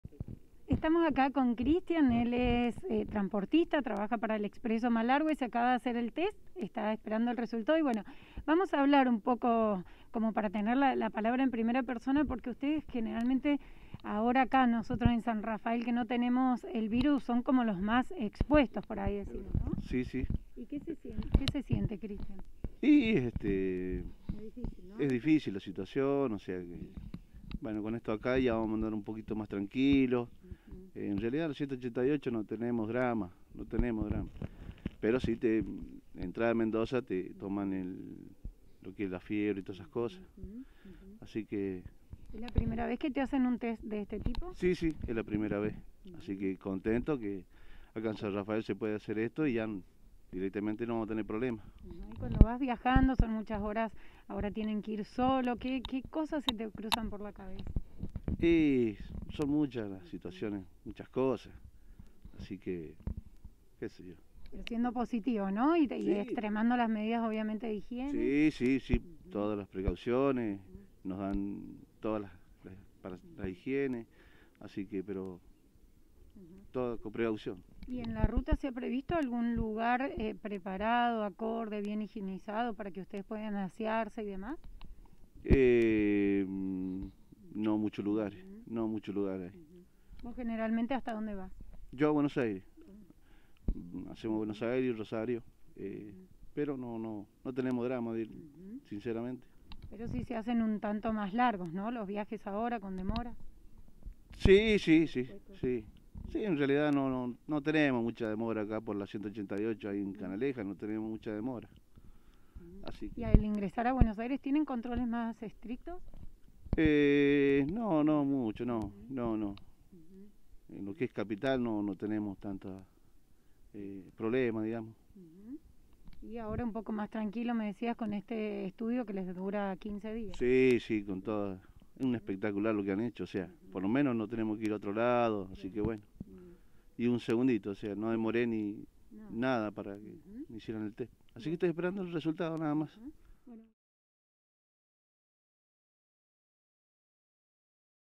Test rápidos de Covid-19 en el Centro de Congresos
Dialogamos con Cristian, uno de los transportistas de Expreso Malargüe, quien brindó su opinión respecto a esta medida